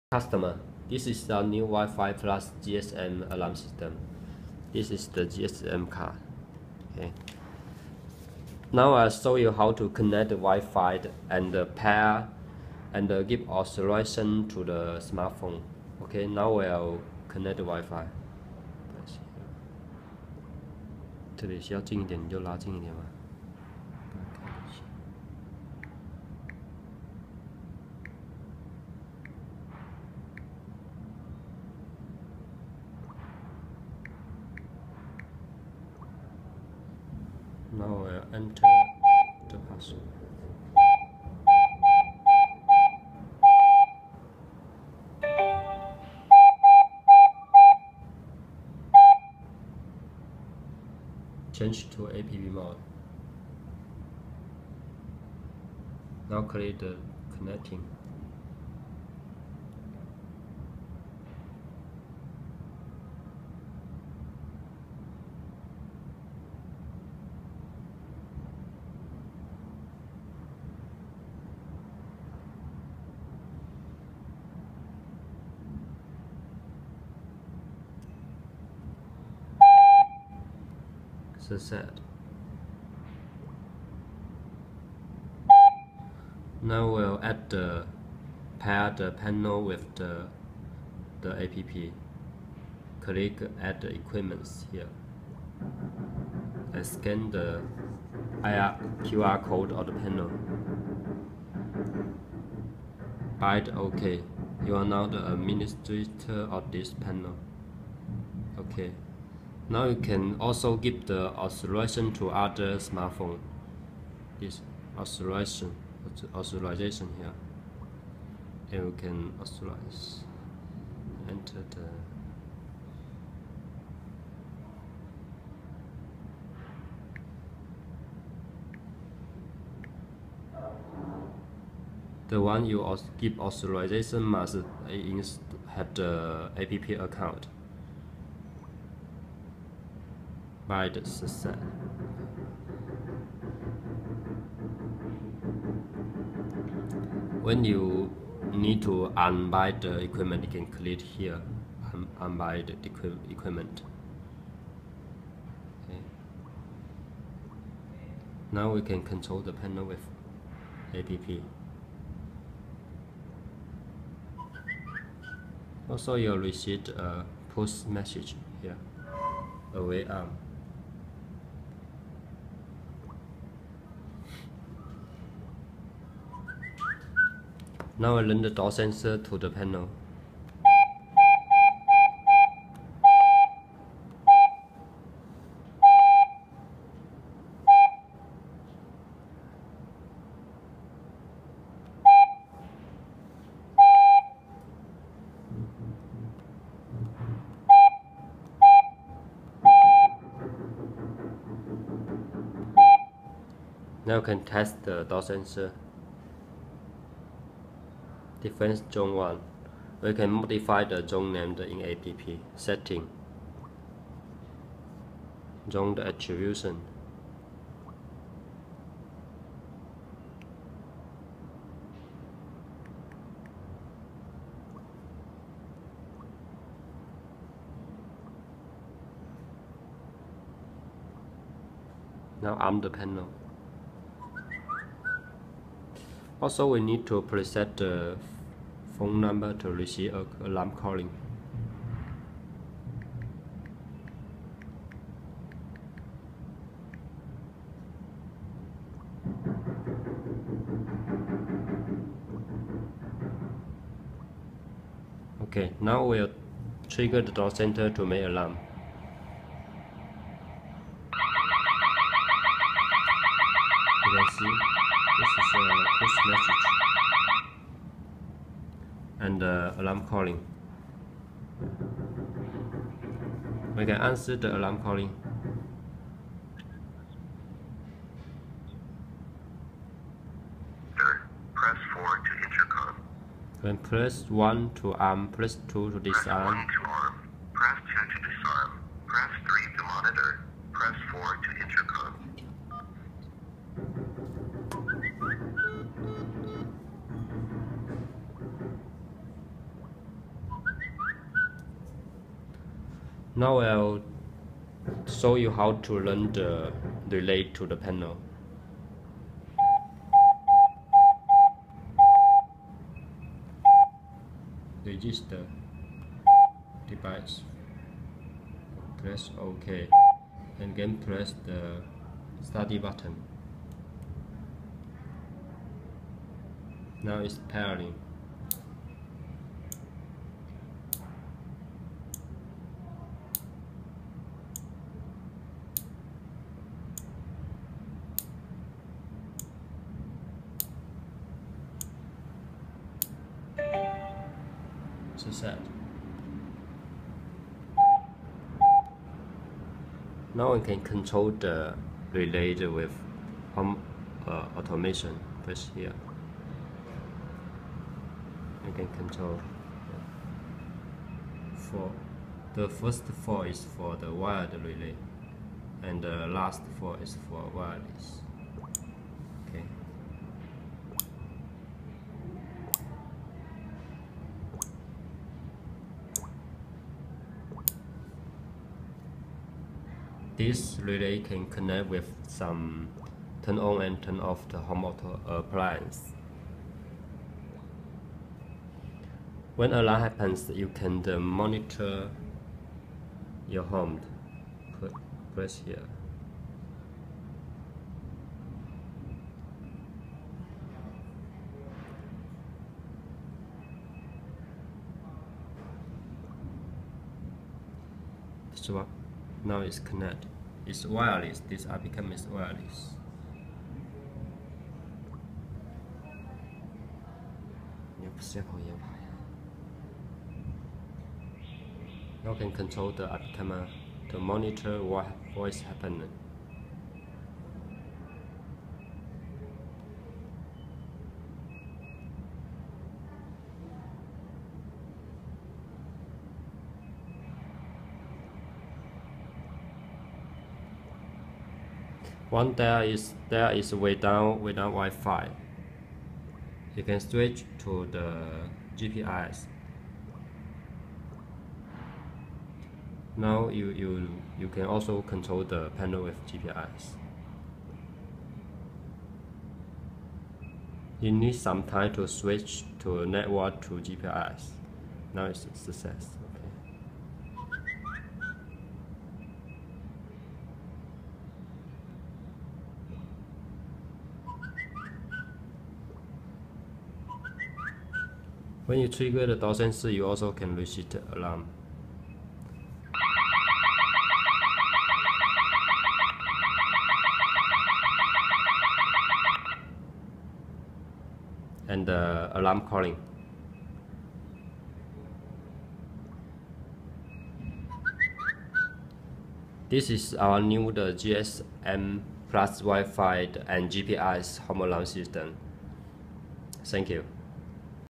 2015 new technology WIFI alarm system, GSM alarm system,GPRS alarm system all in one panel
Factory directly supply WIFI + GSM + GPRS alarm system Welcome to contact with us.
Skype: zhang.david.watch
Main features:

1.      Use Wifi+GSM network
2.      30 wireless zones+8 wired defense zones
3.      Capacitive touch keypad, Words Menu OLED display.
4.      Each zone names can be customized by users
5.      Can store 6 alarm call phone No.+3 alarm Push No.
6.      Support Three zone types: 24H line, 1st line, 2stline.
7.      Arm/alarm Delay for each defense zone. Timed arm/disarm function
8.      support Independent zones function: Once a zone is set to be independent, the zone can only disarm by SMS, remote fobs and keypad is not workable.
9.      Remote voice message function, users can use APP for voice message recording ,alarm host will play this new voice message
10.    Support Monitoring and two-way intercom function.
11.    The host can be used as a hand-free phone.
12.    Information push promptly when AC power lost or recovered.
13.    Information push promptly when detectors in low voltage
14.    Smart zone function: self-checking windows/doors status and chime reminds if door open
16.   Work with IP camera.
17. Workable with RFID wireless keypad
18. Can control 4 wireless relay outputs and wireless sound&strobe siren.